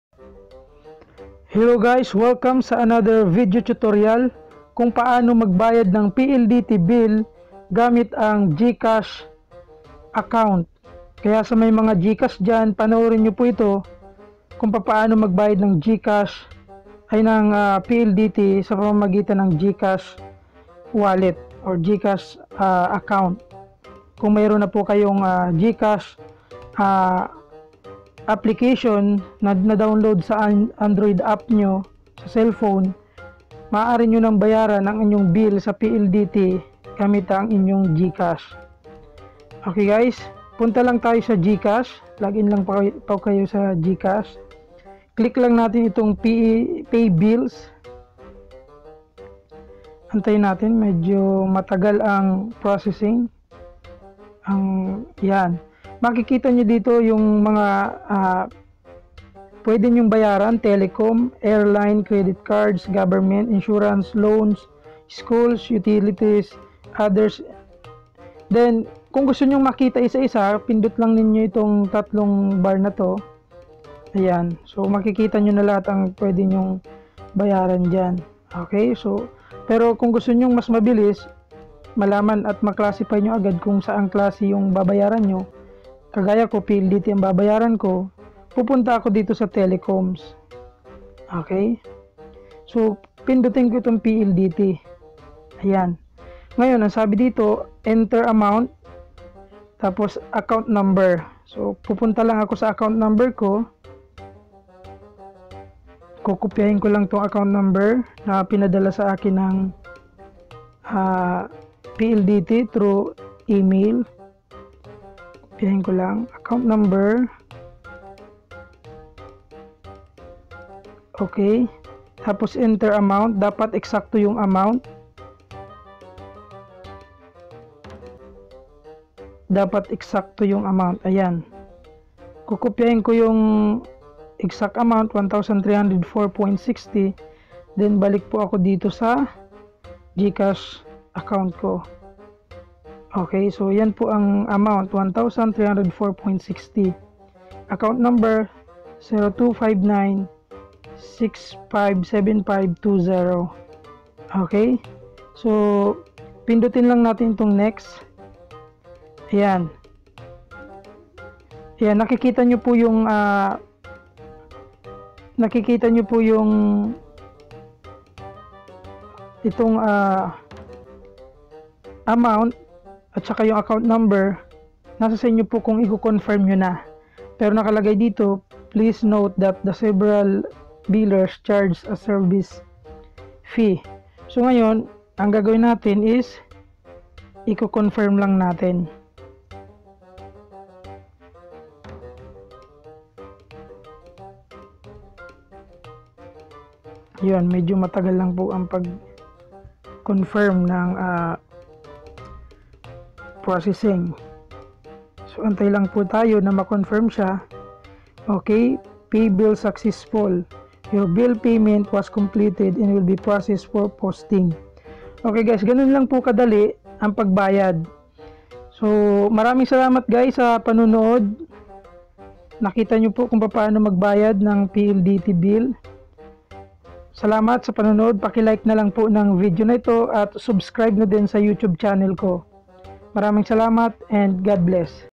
How to pay PLDT BILL using GCASH App (Tagalog)
How to pay PLDT Bill via Gcash android app.

Kung wala ka pang GCASH WALLET Account, register ka muna dito at makakatanggap ka ng PHP 70 worth of freebies for you to try it out: